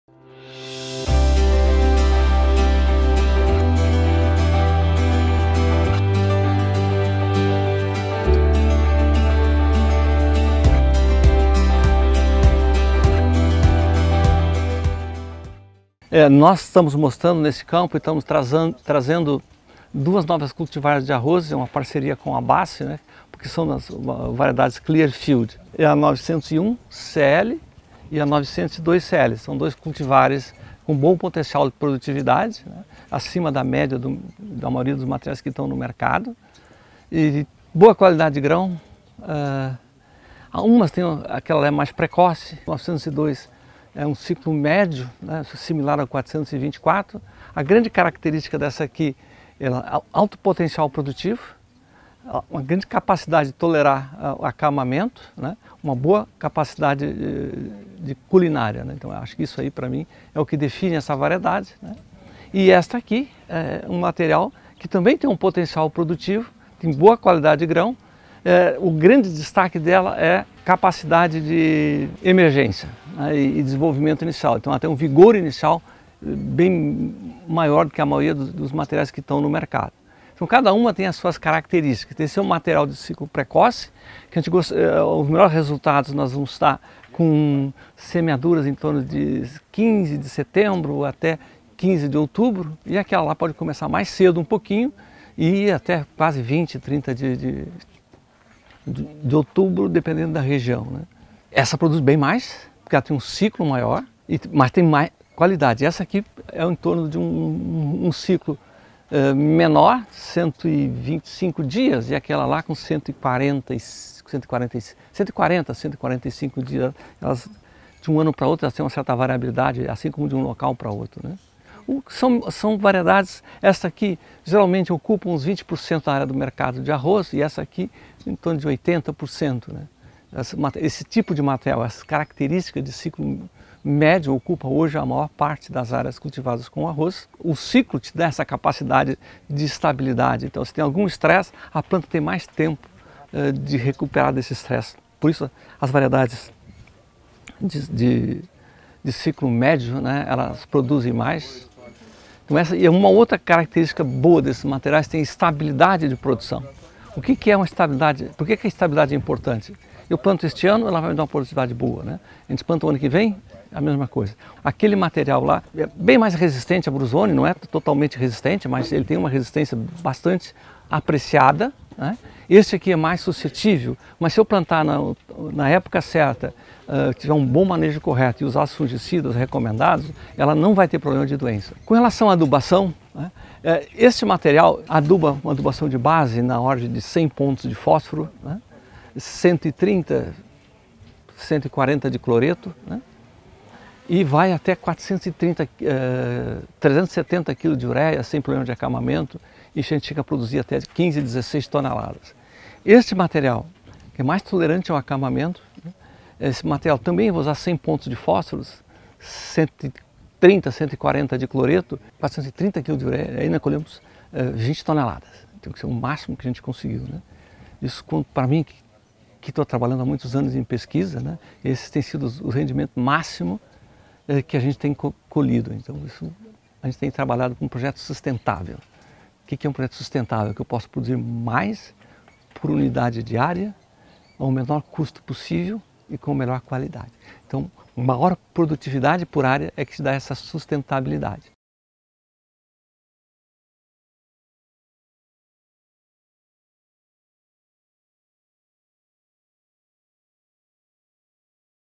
Cultivares de arroz OS 901 CL e OS 902 CL proporcionam sustentabilidade na produção
O pesquisador da Oryza & Soy, Valmir Menezes, apresenta as cultivares de arroz OS 901 CL e OS 902 CL com a tecnologia Clearfield®, que confere as plantas de arroz tolerância a herbicidas no controle do arroz vermelho e de outras importantes plantas invasoras da cultura. As principais características dos materiais são: alto teto produtivo, qualidade de grãos e  tolerância a Brusone. Assista..